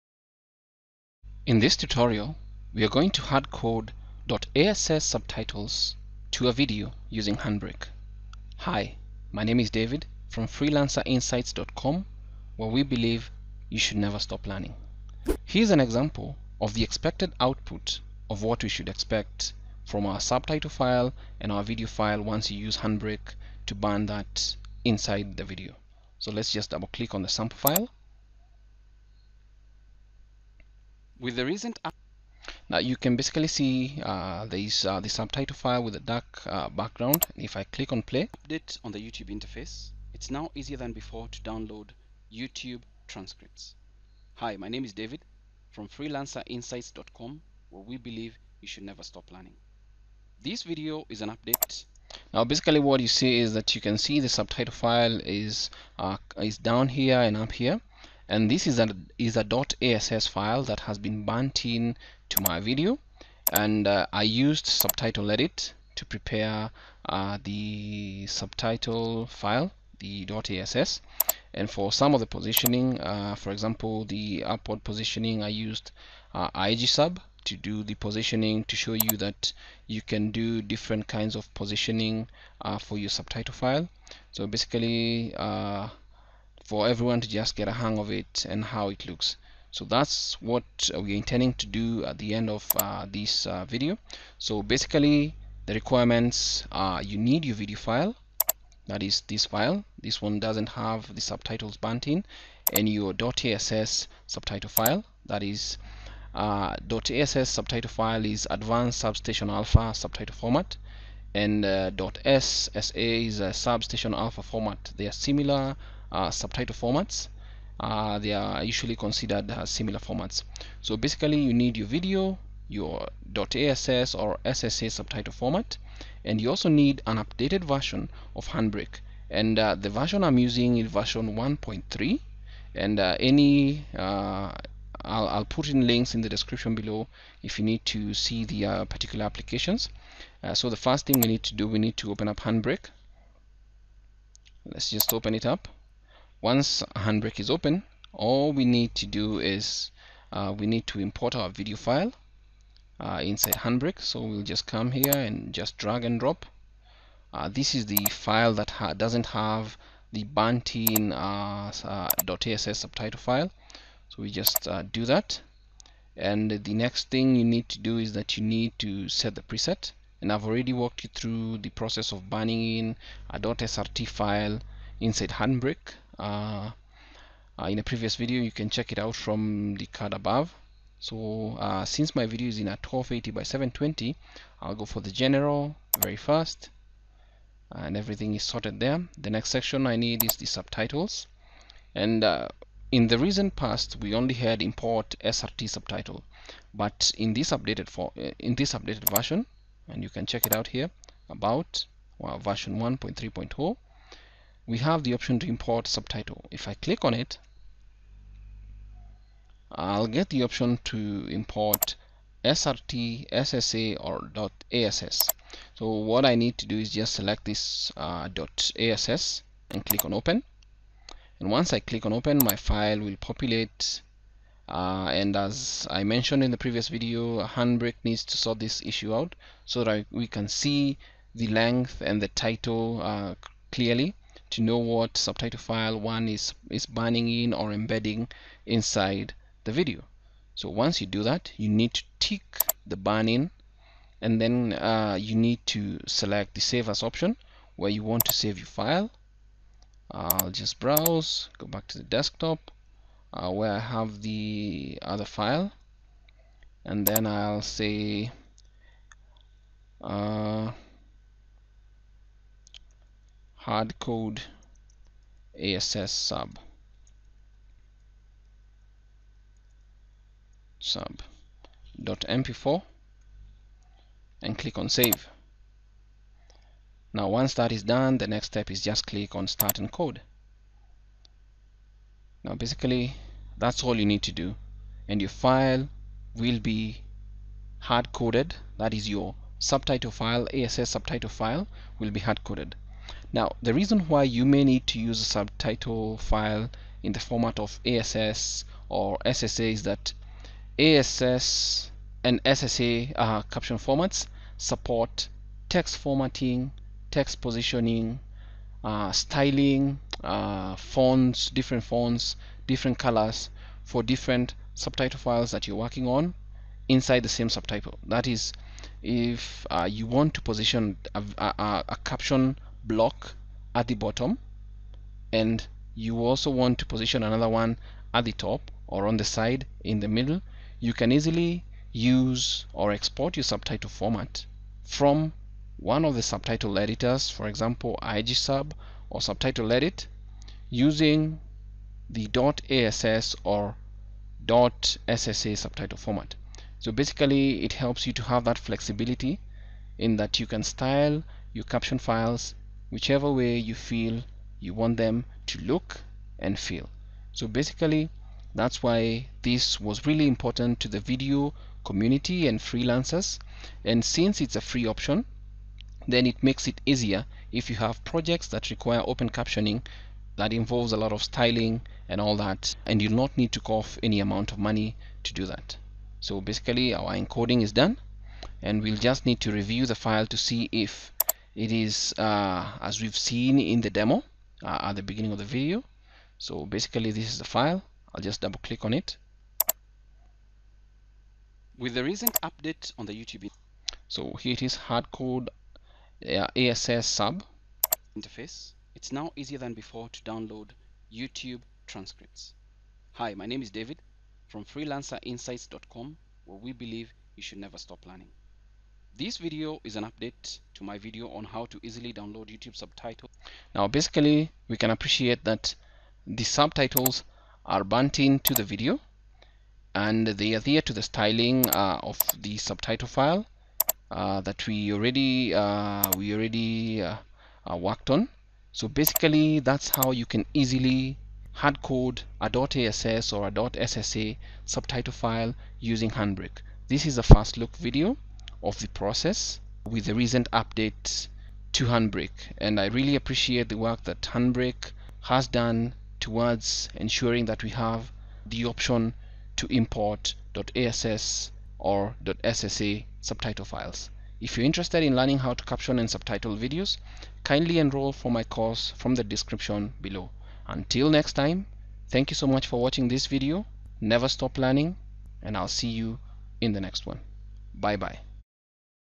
How To Permanently Add SRT, .ASS or .SSA Subtitles To A Video File Using Handbrake
In this tutorial, we're going to Hardcode or burn-in .ASS subtitles to a Video using Handbrake.

With the recent update to Handbrake, to version 1.3.0, it's now possible to burn-in .ASS subtitles directly using Handbrake.

Many people have requested this feature to be added to Handbrake over the years and it's a good thing that the developers are listening.

For this to work, you need your subtitle file (.ASS or .SSA), your video file and an updated version of Handbrake (preferably Handbrake 1.3.0 and above)